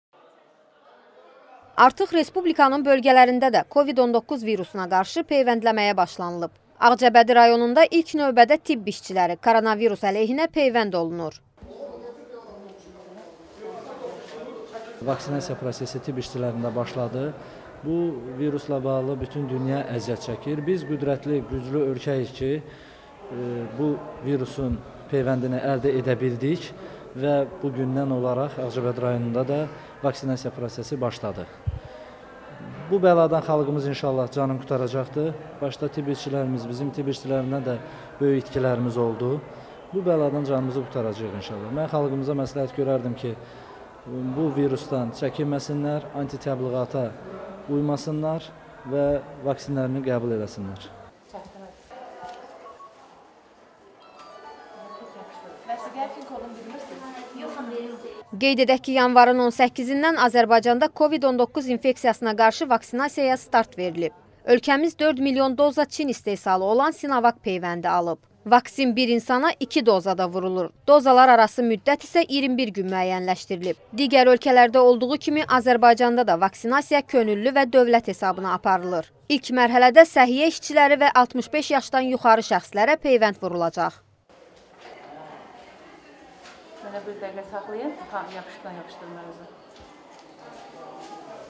Bölgələrdə peyvəndləməyə başlanılıb
Ağcabədidə tibb işçiləri "COVİD-19"a qarşı peyvənd olunur